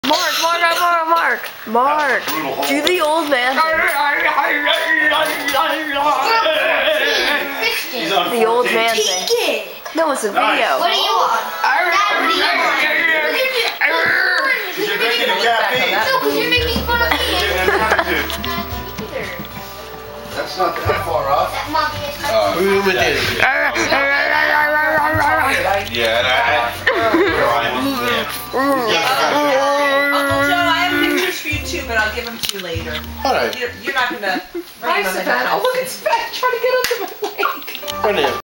My Brother Acting Like A Crazy Old Guy! :D
my brother acting like a crazy old guy with a cane. :D he's just so ridiculous!